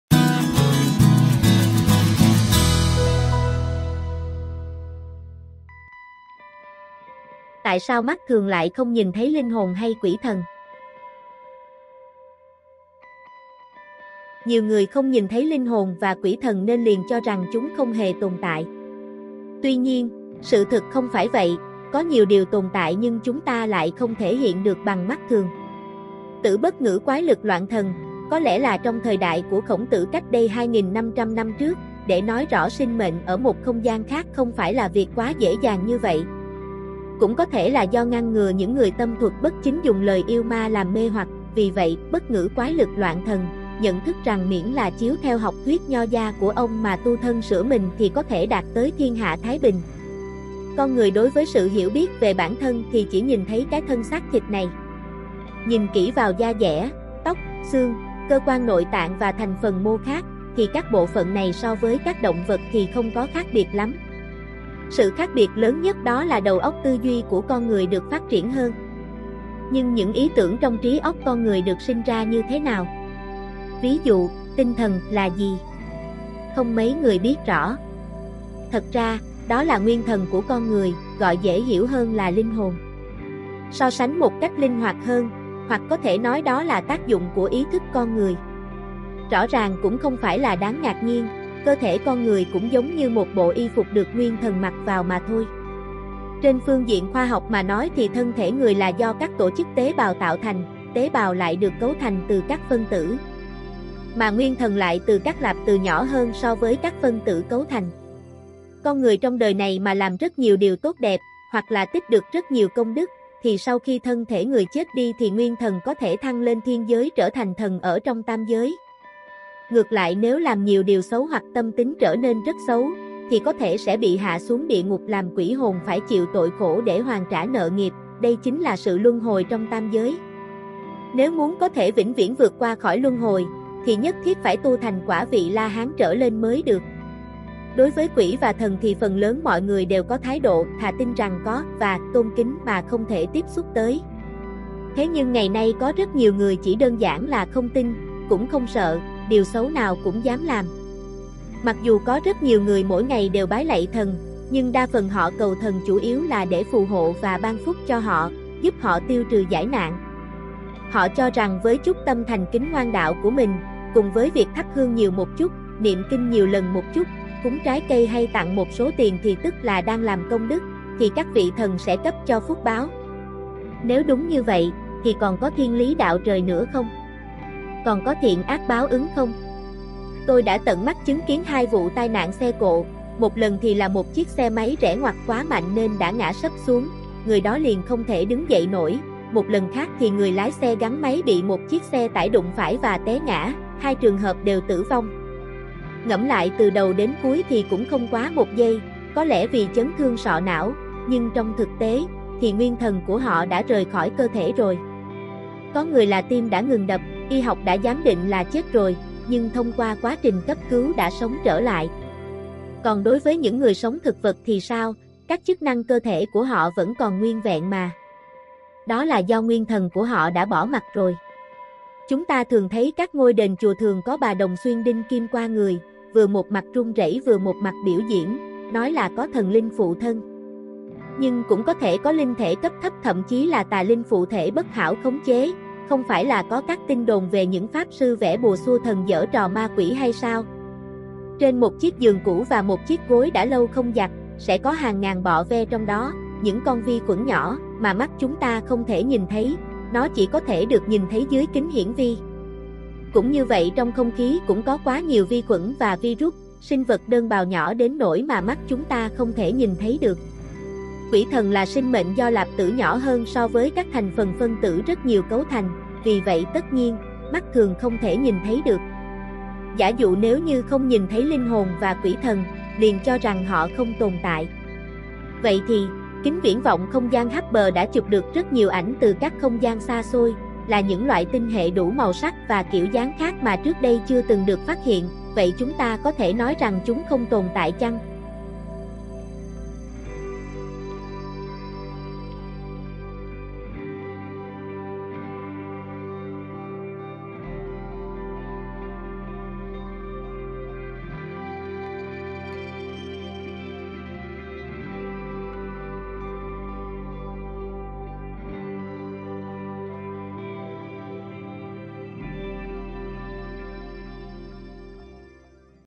Tại sao mắt thường của con người lại không nhìn thấy linh hồn hay quỷ thần
Nhiều người không nhìn thấy linh hồn và quỷ thần nên liền cho rằng chúng không hề tồn tại. Tuy nhiên, sự thực không phải vậy, có nhiều điều tồn tại nhưng chúng ta lại không thể hiện được bằng mắt thường.

► Skype: Tungcom510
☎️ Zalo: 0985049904
© Bản quyền thuộc về Tùng Phạm